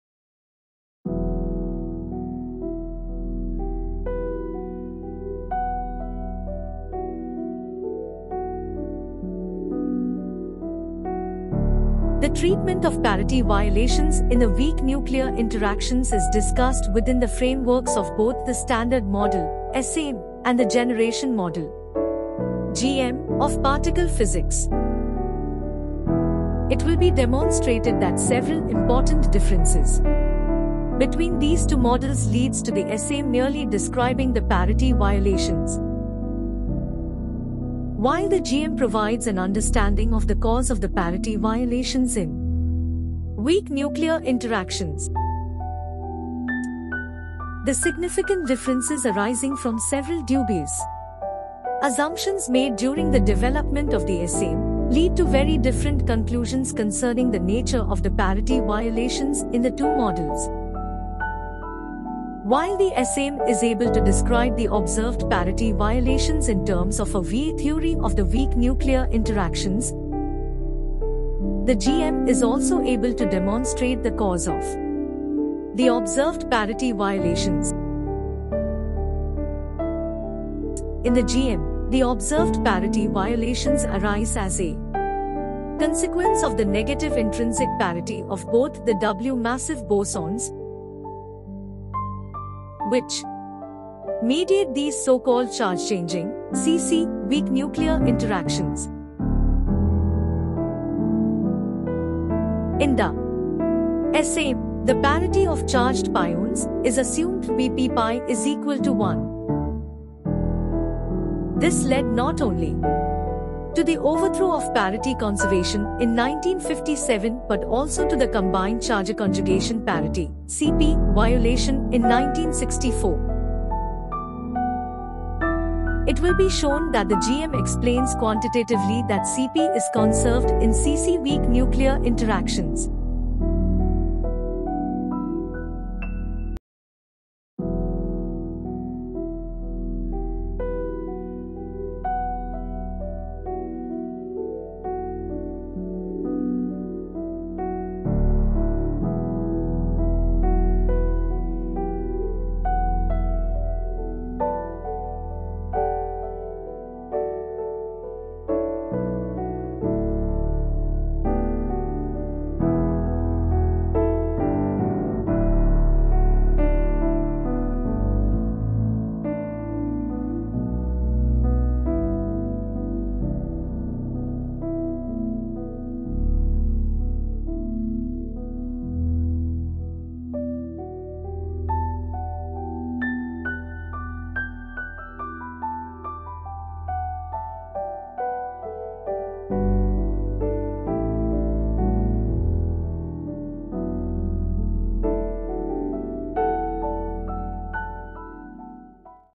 Parity Violation in Weak Nuclear Interactions: A Comparative Analysis within the Framework
Parity Violation in Weak Nuclear Interactions: A Comparative Analysis within the Framework of Standard Model and the Generation Model